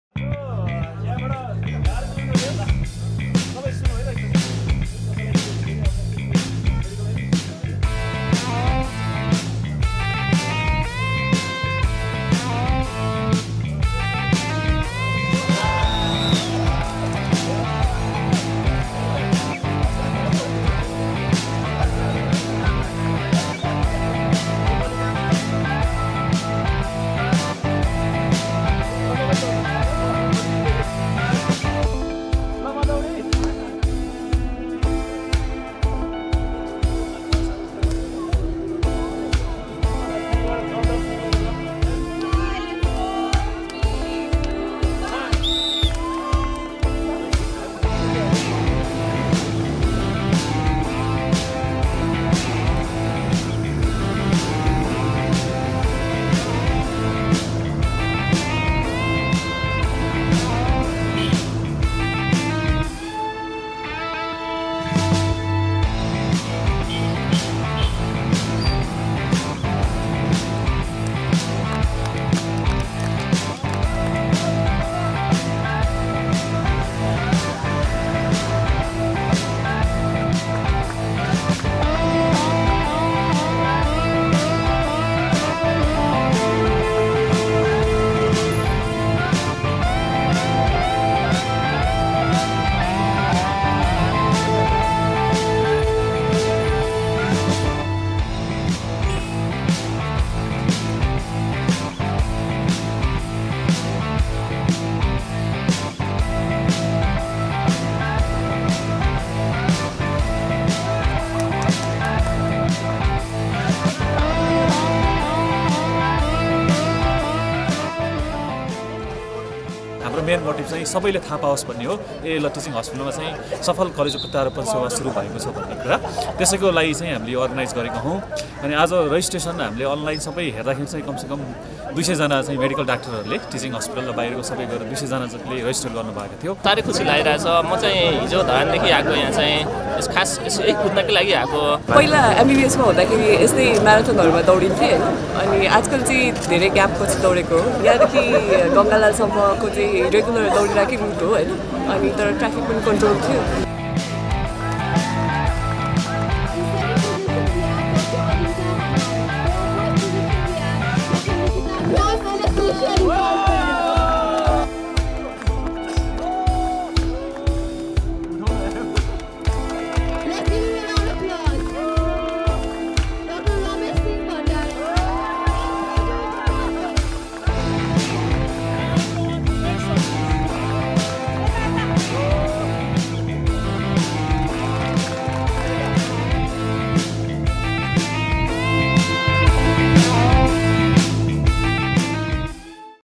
कलोजो प्रयारोपणबारे सन्देश
कलोजो प्रयारोपणबारे सन्देश दिन मिनी म्याराथुन दौड